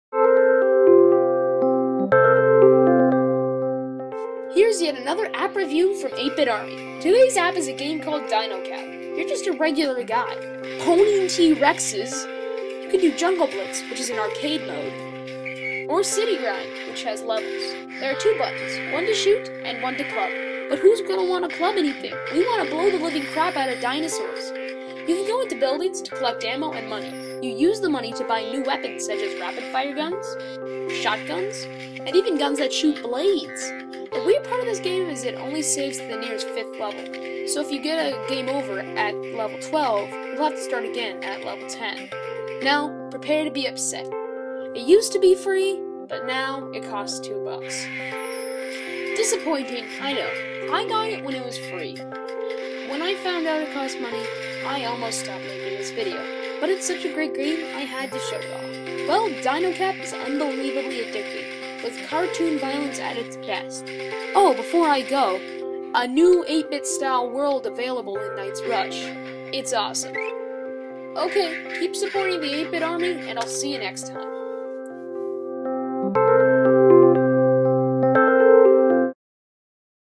Dino Cap Review
A fun cartoon shoot-em up for the iPod that you should definitely check out.